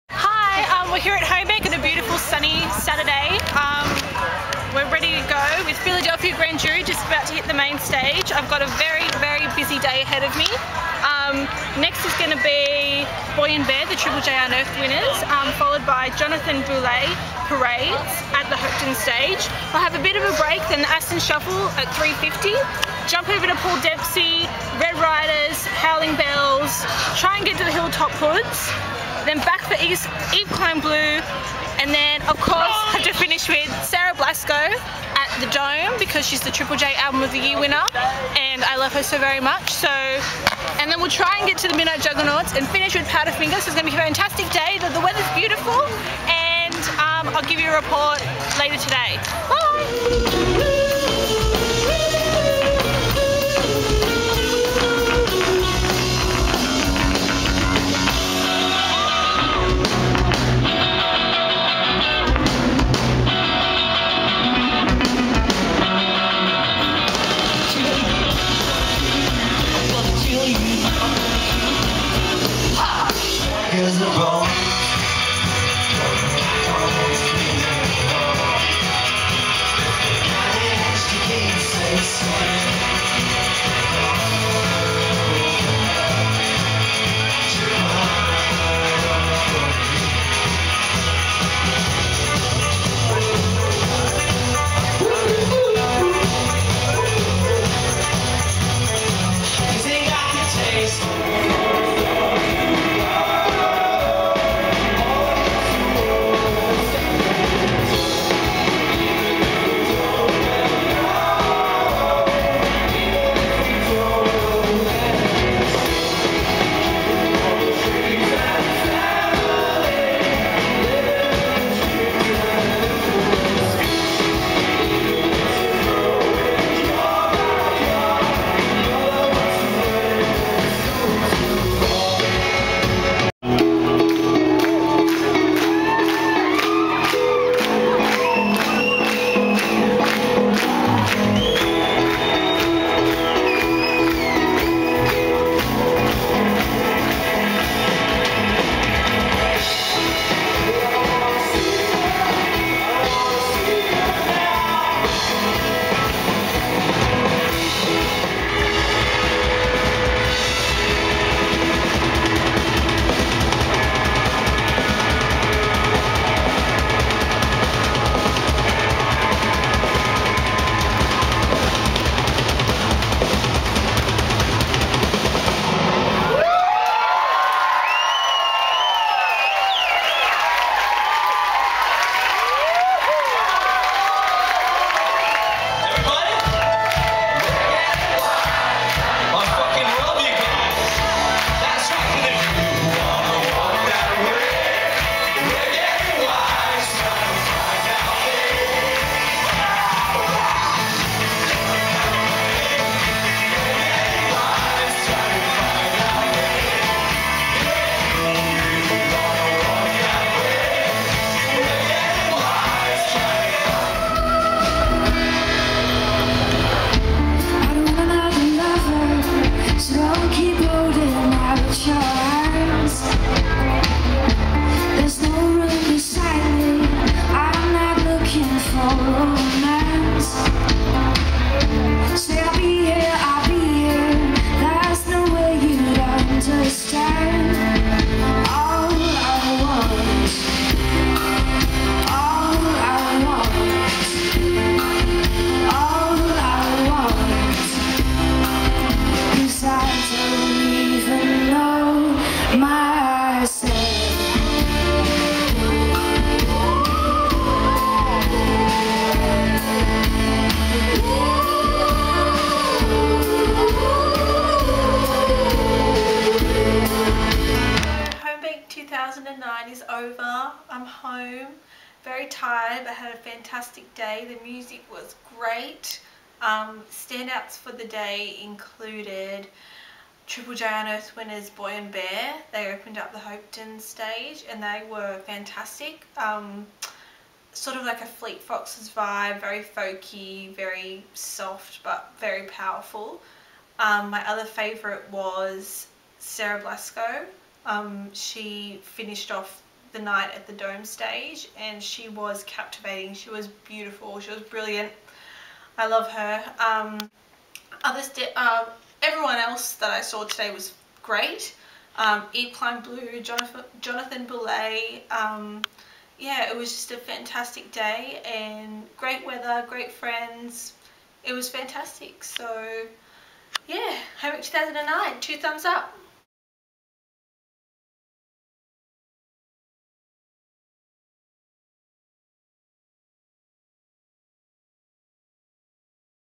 Homebake2009.m4v
My day at Homebake 2009 @ the Domain on Saturday December 5, 2009. I managed to see twelve bands on the day including Philadelphia Grand Jury, Boy & Bear, The Middle East, Jonathan Boulet, Parades, The Aston Shuffle, Phrase, Red Riders, Hilltop Hoods, Yves Klein Blue, Sarah Blasko and Powerfinger. I captured most of these on camera to give you a glimpse of the day- I'm sure you can see from my transition from morning to night that I had a massive day but it was a fantastic day with GREAT music. I'll be posting a blog soon with a more detailed account of my day.